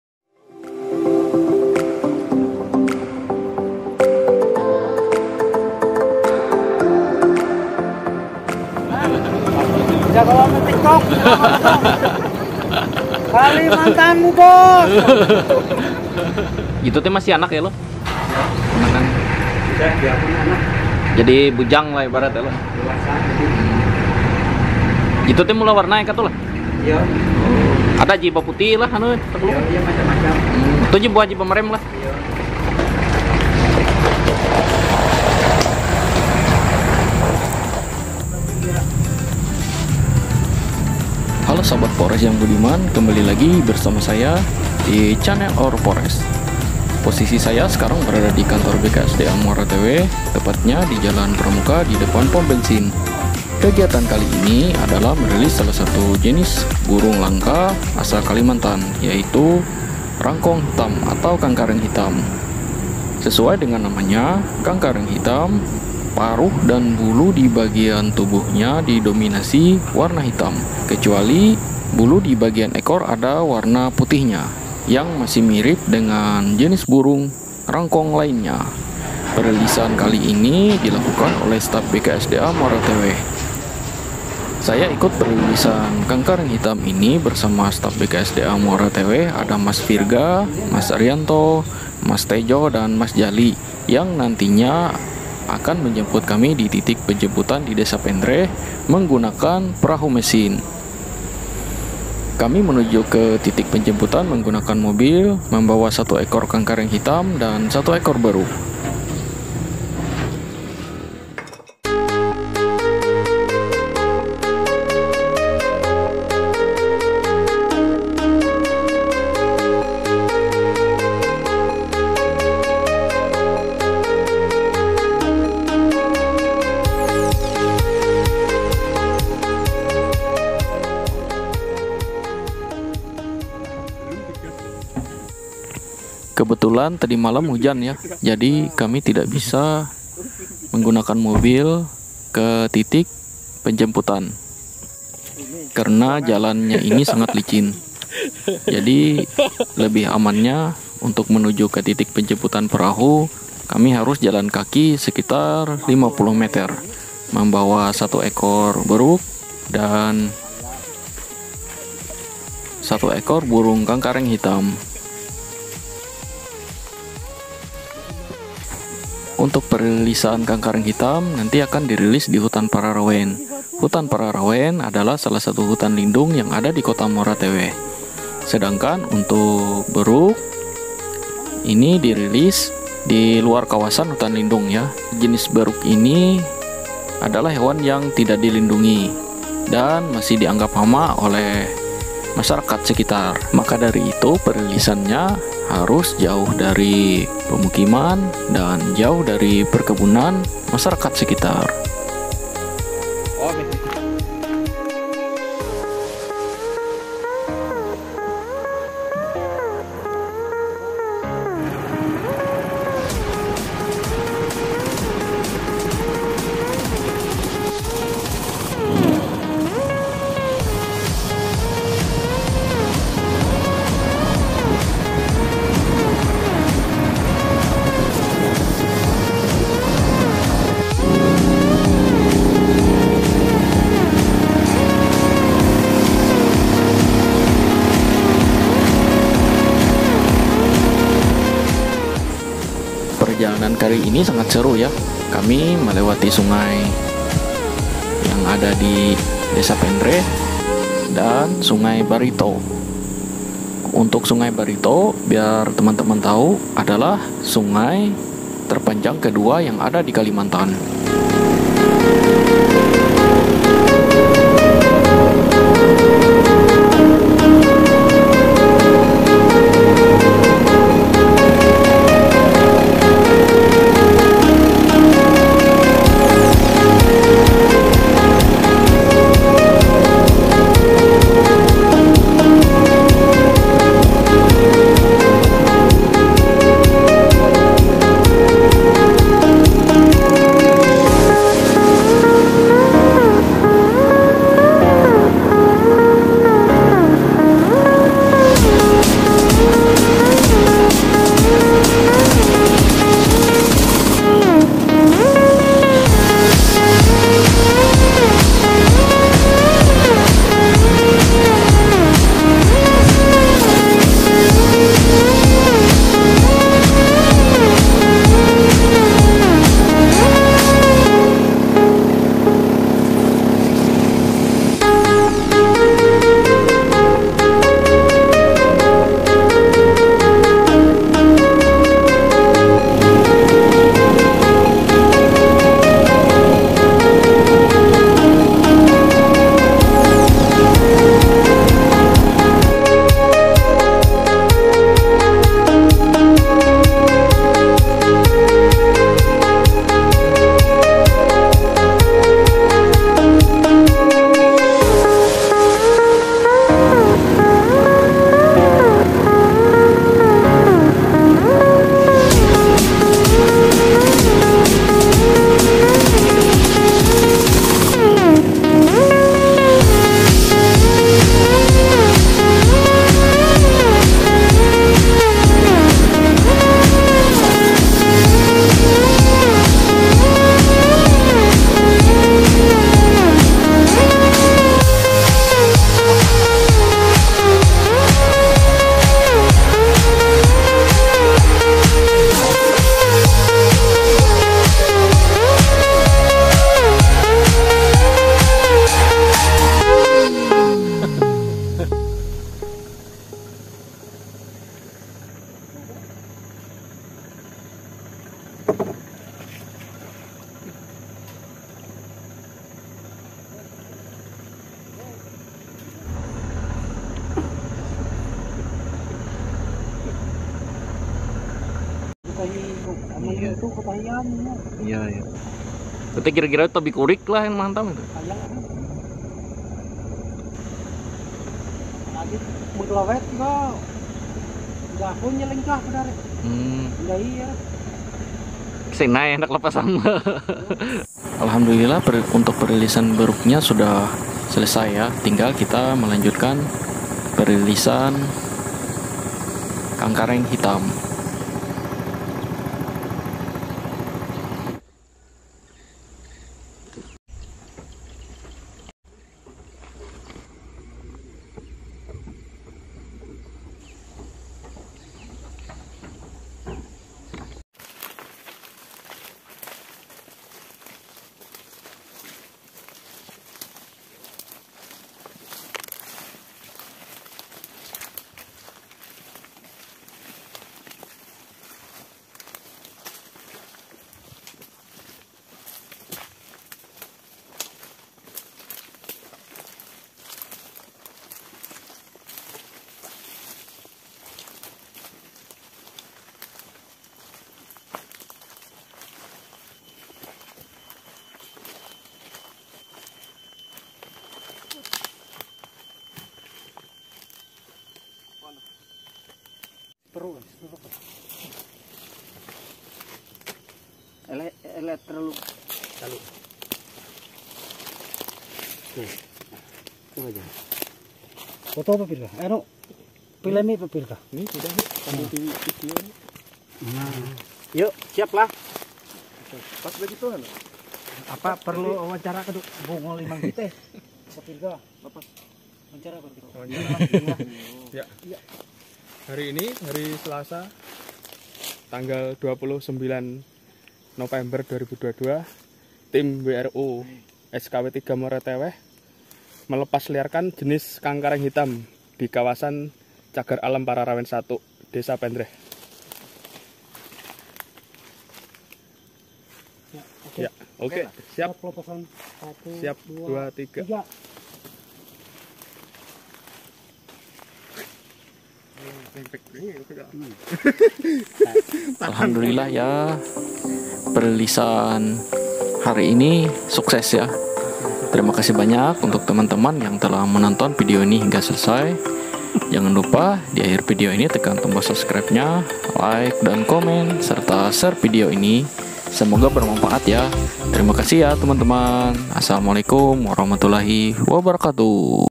Kangkareng Hitam Dirilis Kealam Liar
perilisan burung Kangkareng hitam oleh staf bksda Muara Teweh

tujuan video ini adalah hiburan yang mendidik, semoga dengan adanya video ini masyarakat tidak menebang pohon sembarangan,merusak hutan dan isinya
terima kasih sudah berkunjung di channel our forest, salam lestari dan konservasi